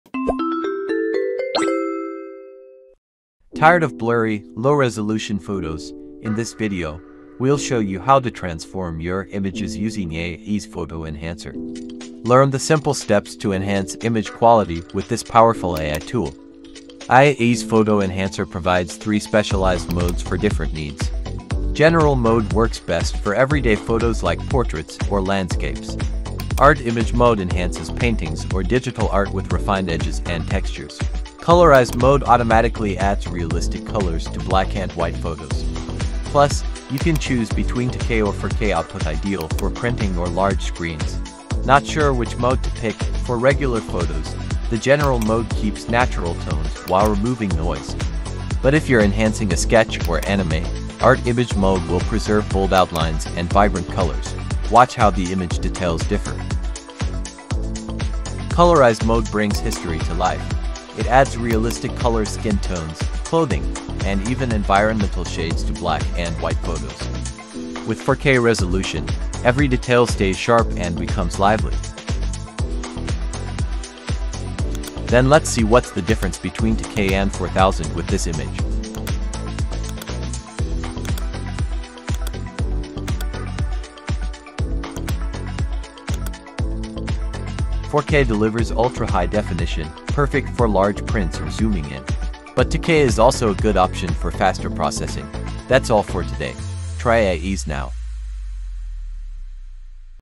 How to Instantly Enhance Your Photos with AI | Free Online Photo Enhancer
Say goodbye to blurry, dull photos!
In this quick tutorial, learn how to use AI Ease’s free Photo Enhancer to upgrade your image quality instantly — improve sharpness, resolution, brightness & more, all online and hassle-free.

Perfect for social media, professional shots, or bringing old photos back to life!